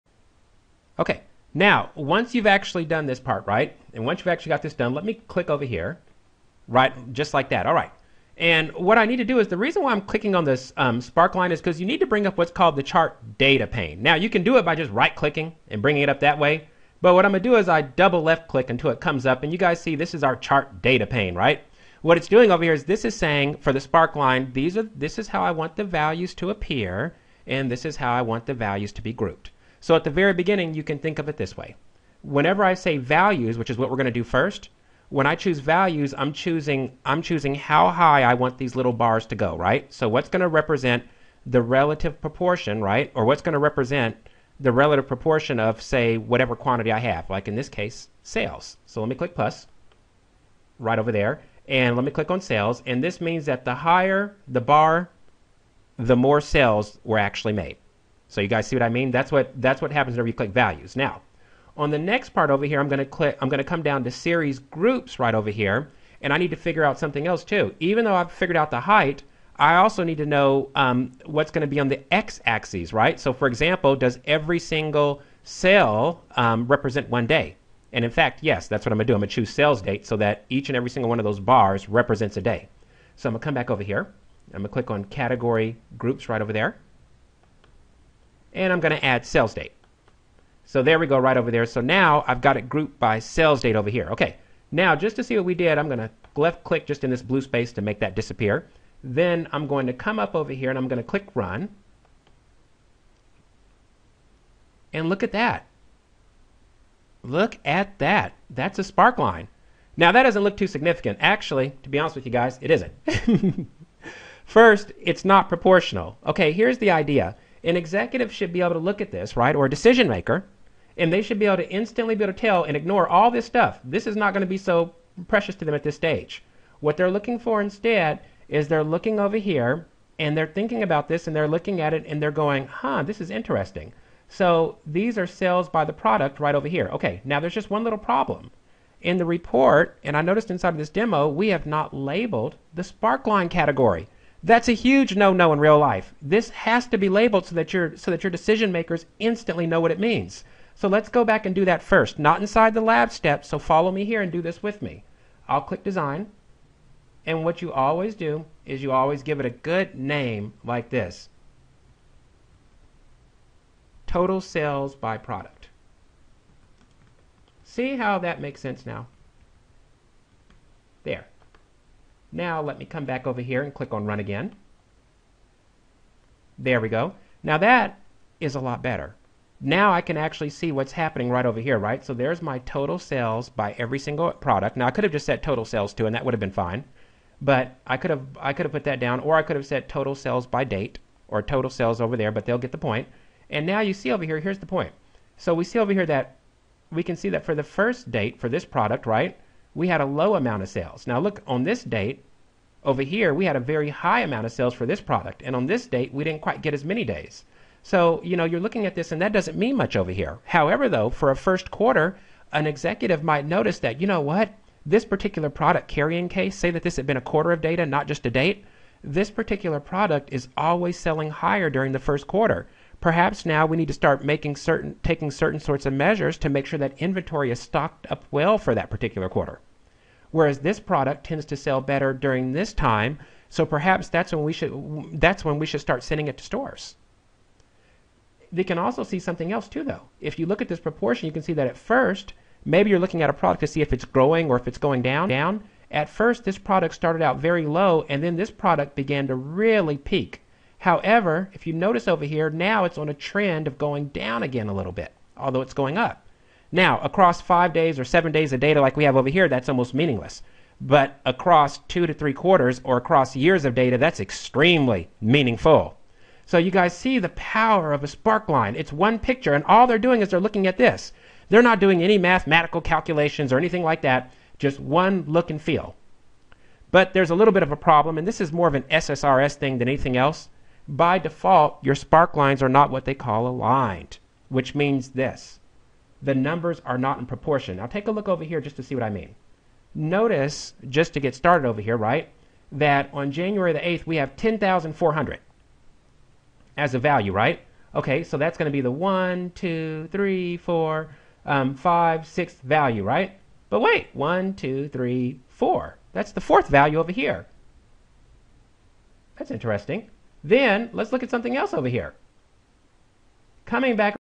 Report Builder for SQL Server 2012 Part 8C: Sparklines, why & how to create them
This is one of the most underestimated and least understood features within BI, I find.  Edward Tufte,my hero and one of the fathers of data visualization, coined this term for a small graphic that could cause major change within an organization when presented right.  This small graphic could light a fire of change within an organization leading to more profitability and better decision-making.  A -spark-, so to speak as he so eloquently coined the term.  Let's see how to create them.